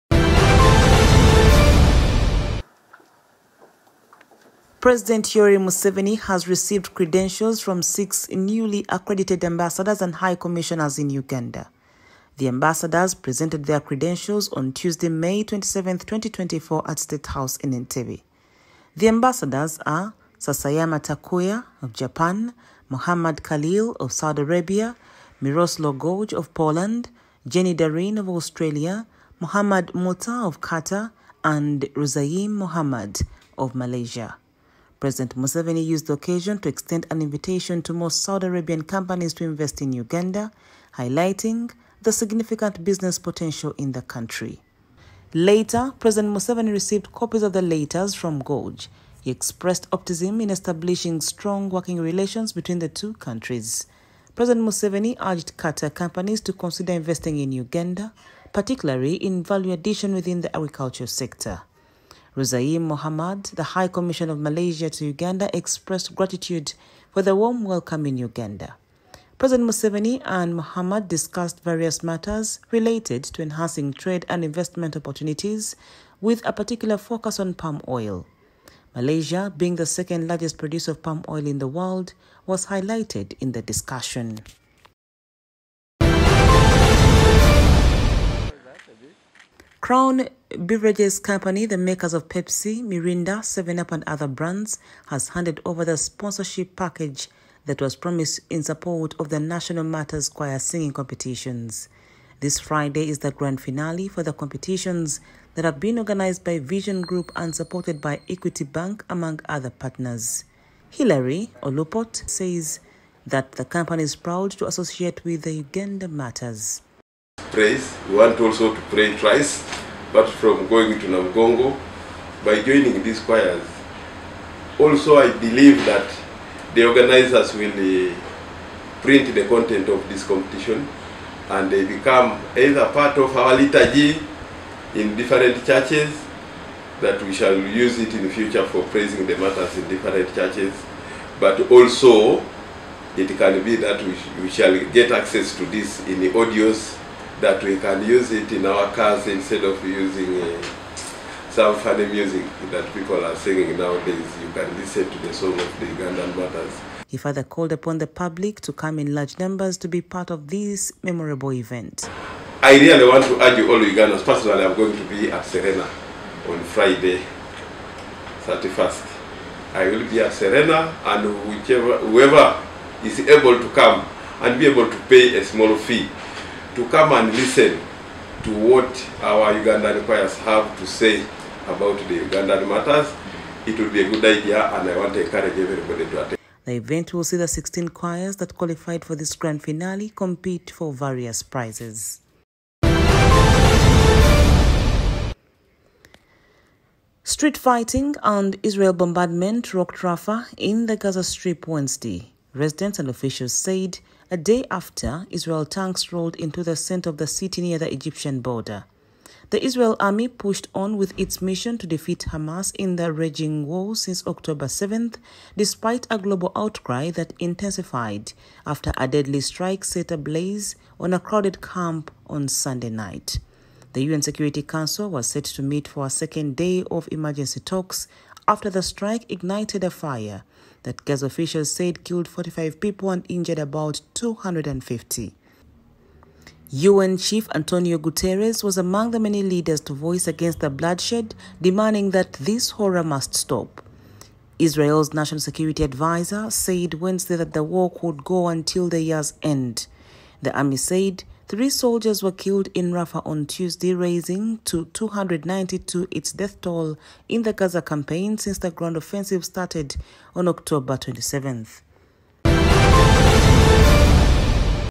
Morning DigitalDigest 30.05.2024
‣ Museveni welcomes new ambassadors
‣ Uganda martyrs choir singing competition
‣ Israeli strikes rock Gaza's Rafah